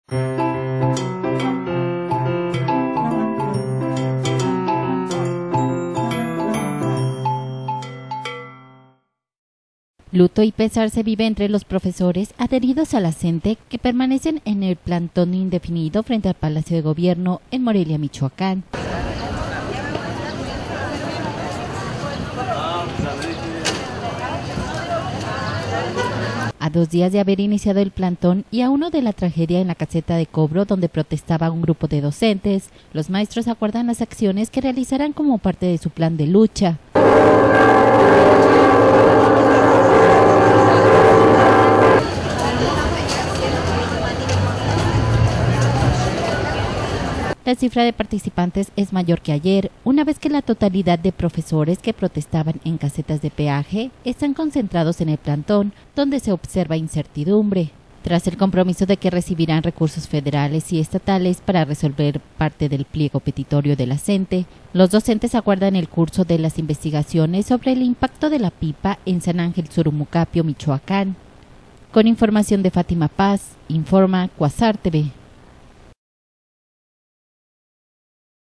Luto y pesar en el plantón de la CNTE, en Morelia
Luto y pesar se viven entre los profesores adheridos a la CNTE que permanecen en plantón indefinido frente al Palacio de Gobierno, en Morelia, Michoacán...